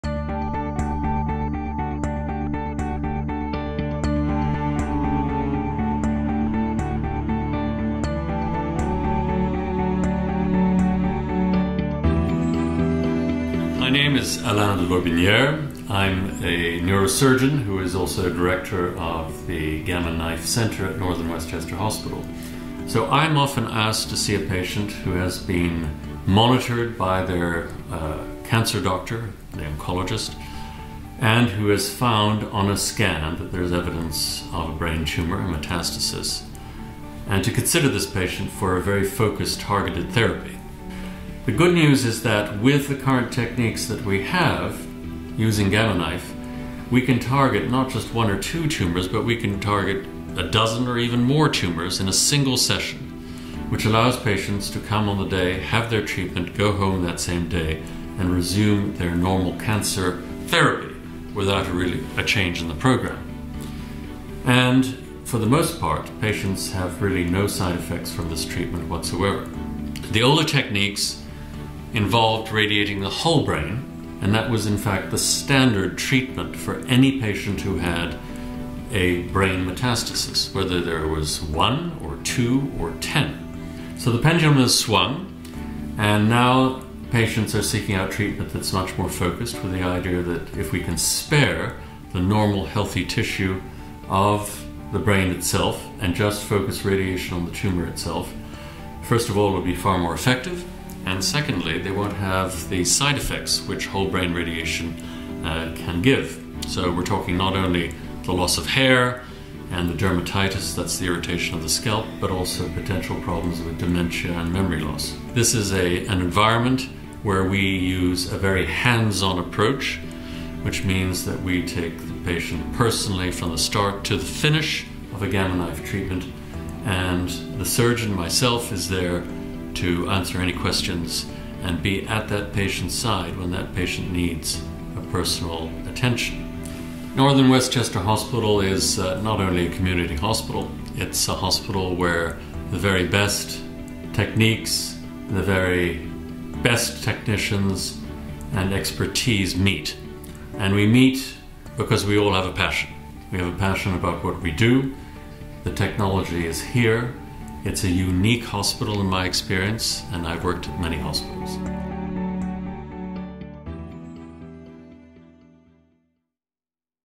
Advanced Treatment for Metastatic Brain Cancer: Dr. Lotbinière
The Gamma Knife is not a knife at all, but a highly sophisticated beam of radiation that hyper-targets brain tumors. It is a non-invasive treatment.  Learn more about Gamma Knife "surgery" from Alain de Lotbinière, MD, CM, FACS, FRCSC, Director of the Gammy Knife Surgery Center at Northern Westchester Hospital, Mt. Kisco, New York